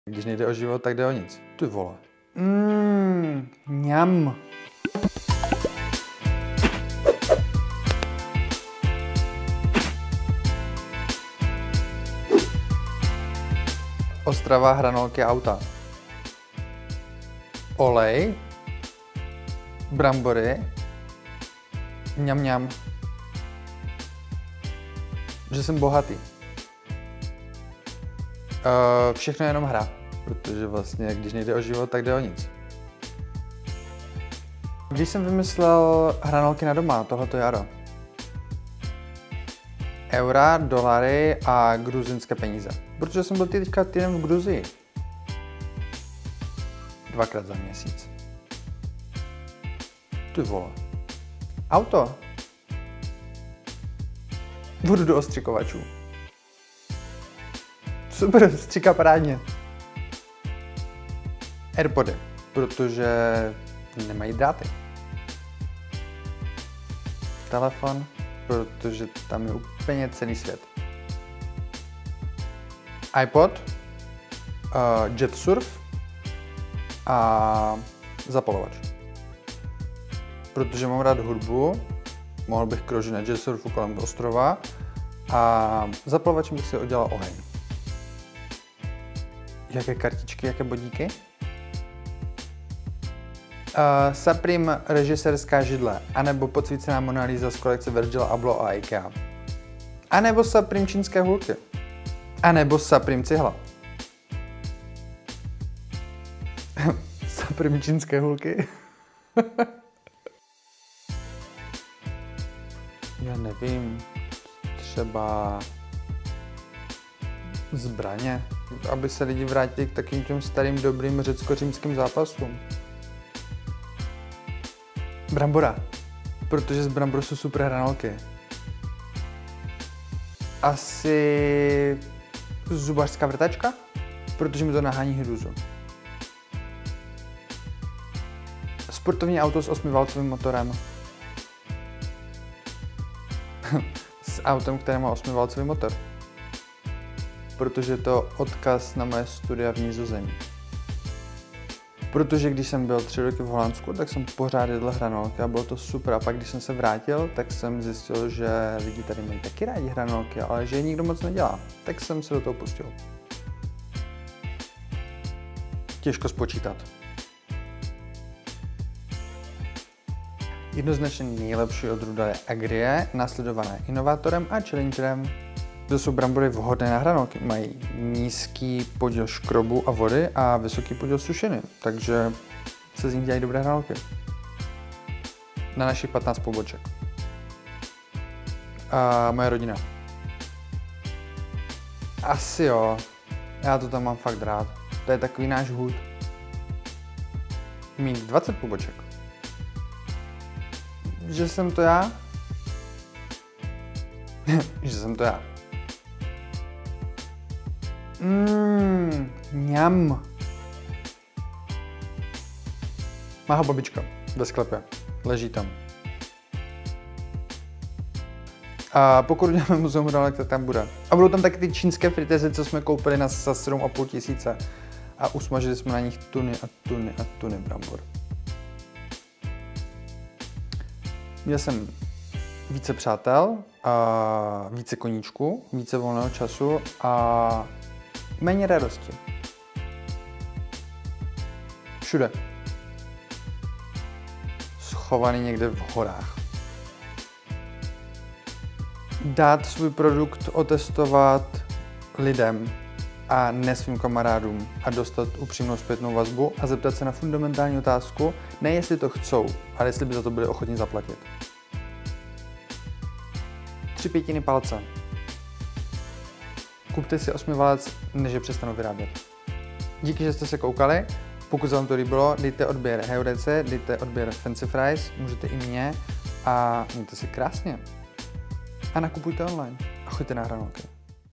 René Müller | Kecy a věci #25
UPDATE: René Muller už sice s FÆNCY FRIES nemá nic společného, ale když měl, takhle o nich mluvil. Poslechněte si dost možná poslední videorozhovor, který v jejich vedení poskytl.

Doufáme, že se vám video líbilo. Pokud ano, nezapomeňte dát like a odebírat náš kanál.

Pamatujte, že na život se musí chytře!